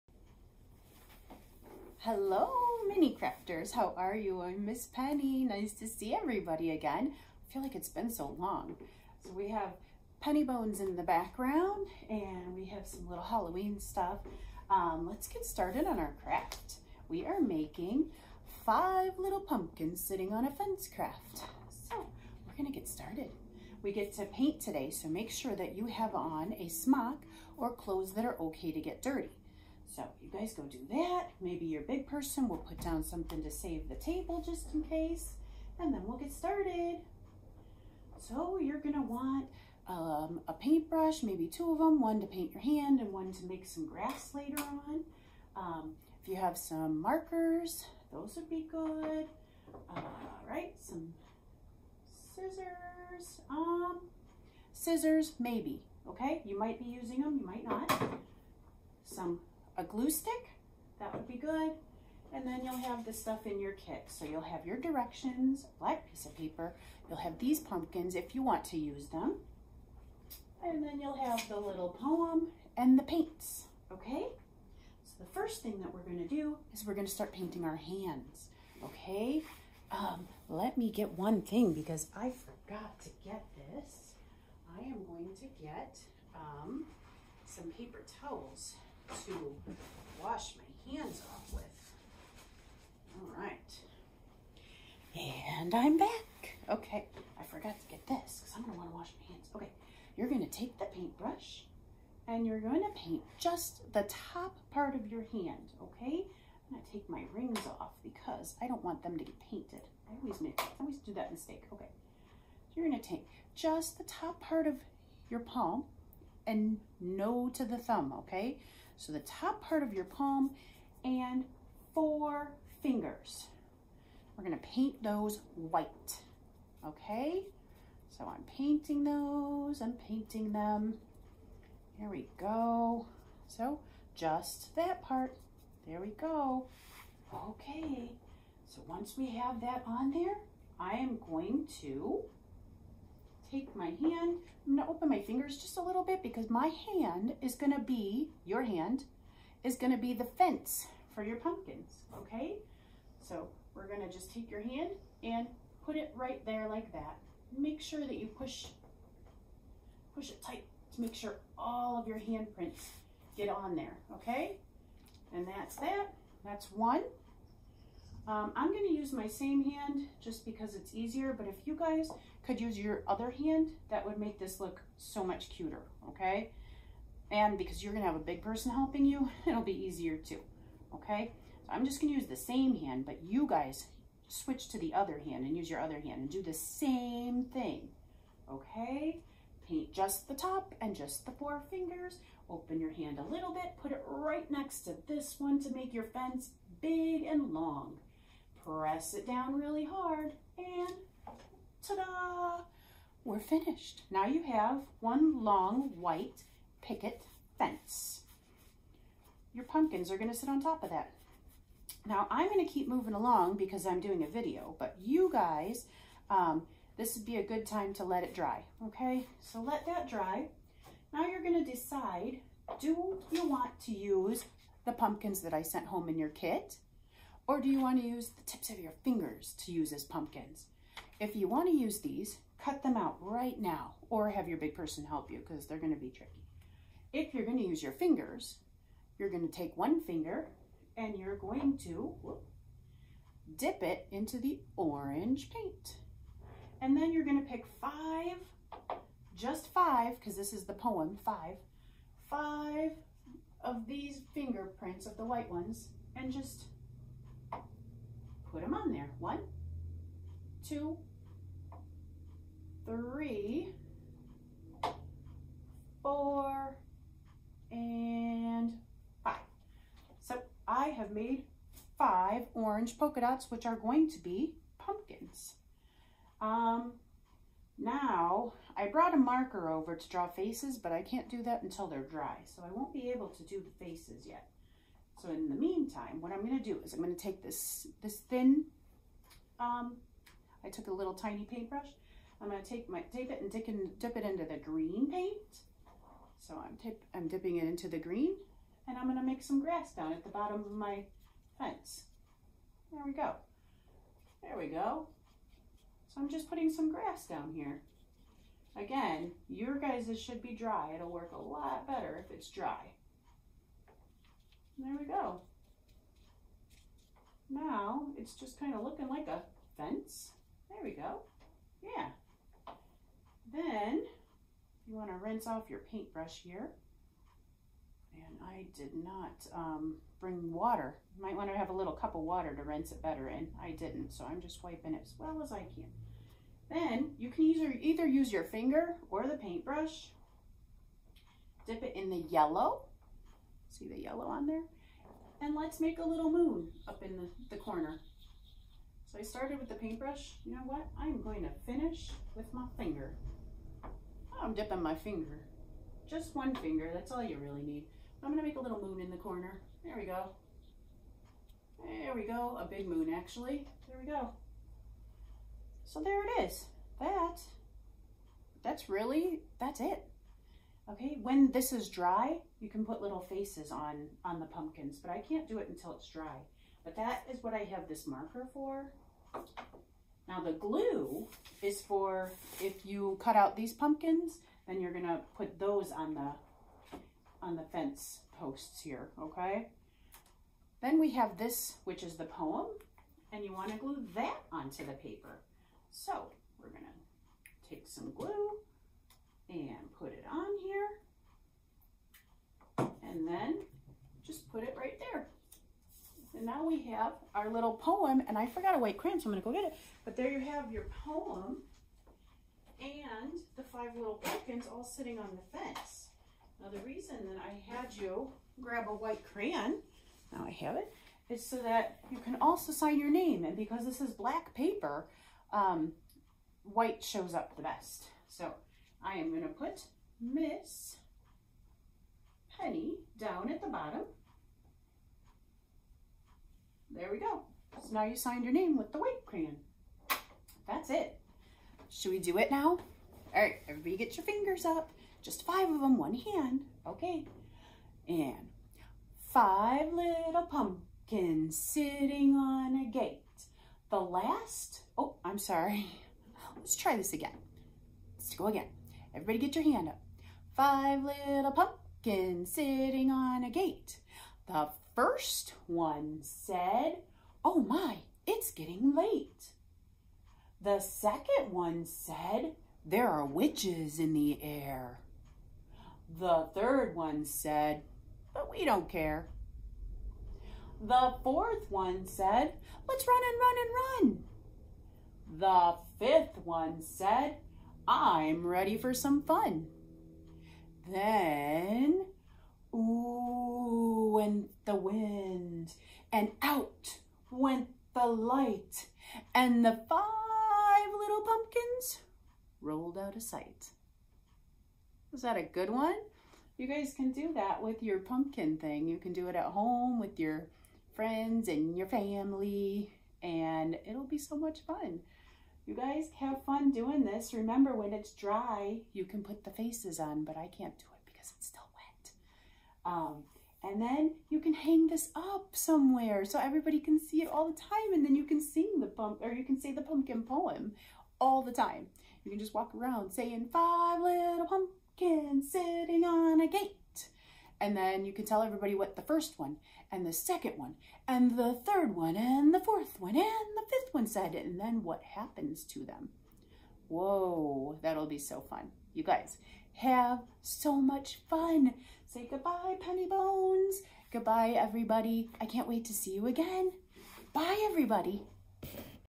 Marengo-Union Library District Mini Crafters 10/18/2021
Join Miss Penny for a fun handprint pumpkin craft. You will paint your fingers (but not your thumb) with white paint to make a fence for your fingerprint pumpkins.
 Included in your craft kit you will find written directions, washable paint (white,yellow, orange, and green). Pumpkin templates are included if you rather use them instead of your fingertips to make pumpkins,  Once your project is dry get ready to recite pumpkin rhyme.